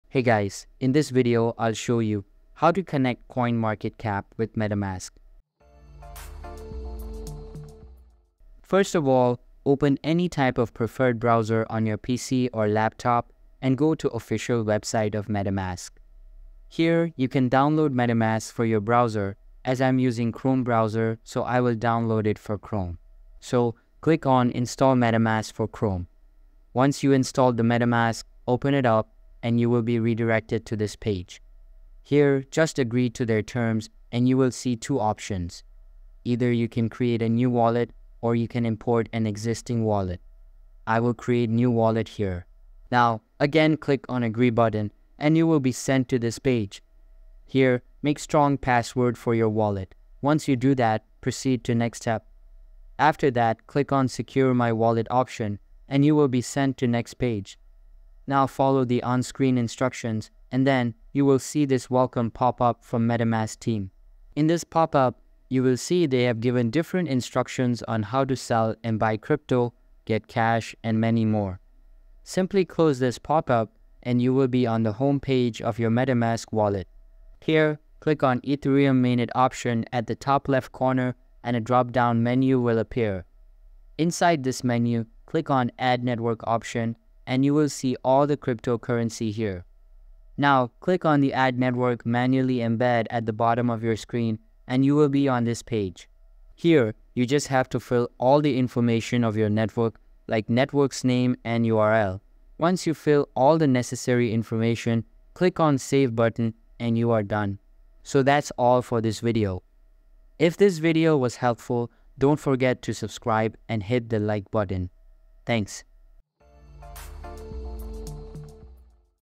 How To Link / Connect CoinMarketCap With MetaMask | Quick Tutorial (2025)
How To Link / Connect Coinmarketcap With MetaMask | Quiick Tutorial (2025)

👉 In this tutorial video, we will guide you on how to seamlessly link CoinMarketCap with MetaMask. Learn how to effortlessly connect CoinMarketCap to MetaMask and establish a smooth integration between the two platforms. Follow our step-by-step instructions to successfully link MetaMask with CoinMarketCap and enhance your cryptocurrency trading experience.